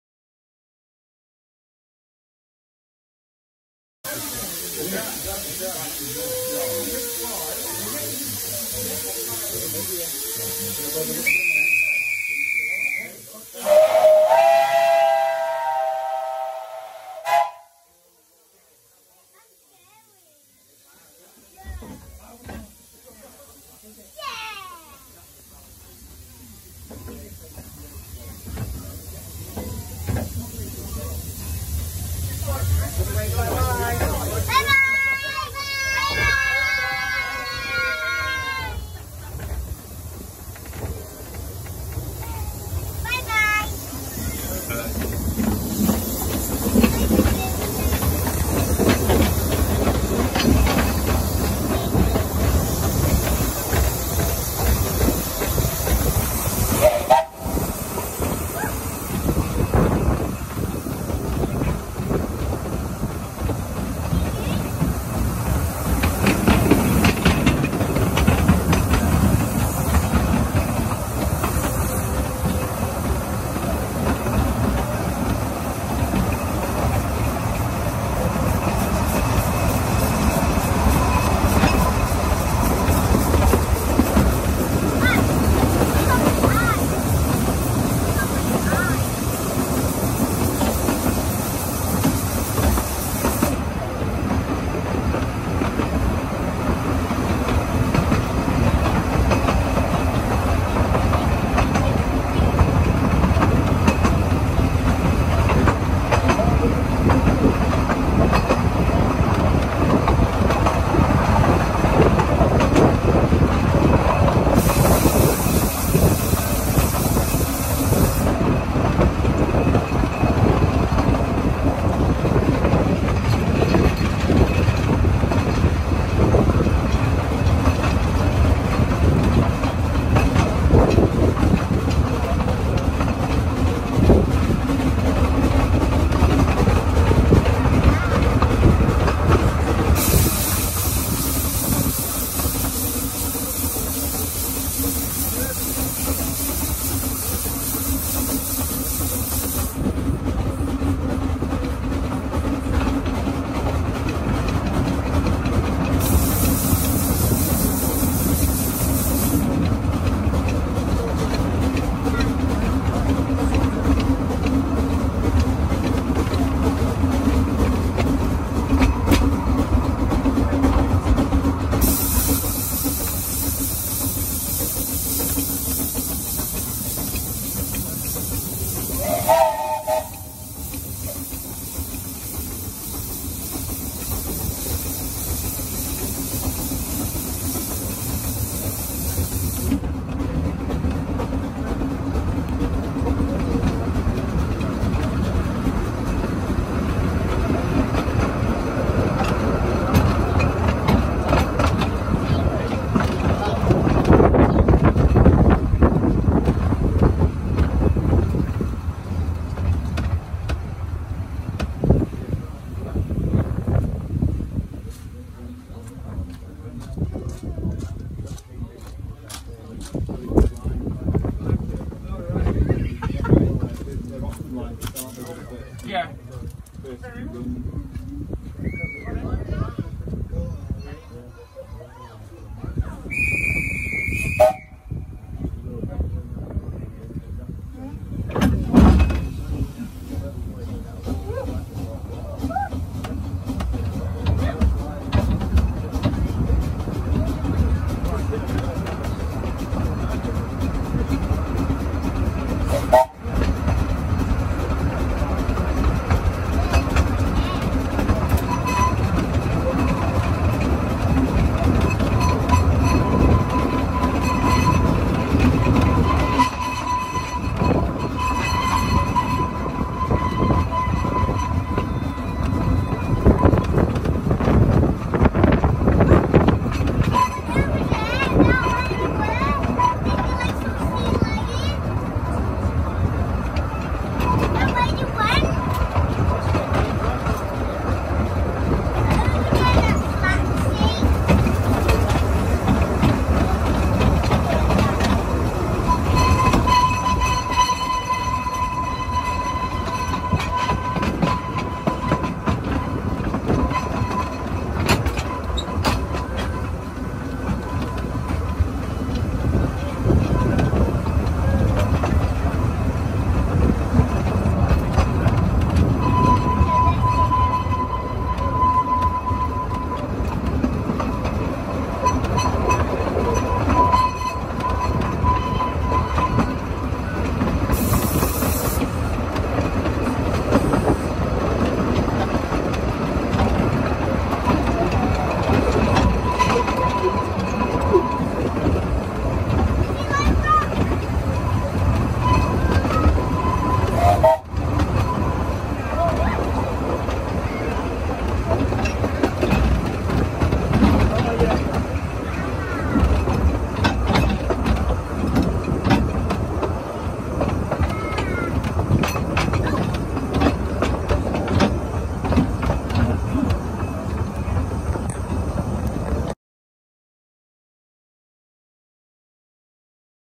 Riding the Groudle Glen Railway - End to End in Six Minutes...
Ride the whole length of the Isle of Man's 2ft gauge Groudle Railway in six minutes - taken on Sunday 5 July 2020 11:00am with loco 'Sea Lion' in charge. Handheld iPhone X Max with full wind effects on clifftop sections, but it can be quite windy on this bit of the line. Enjoy... Cheers

David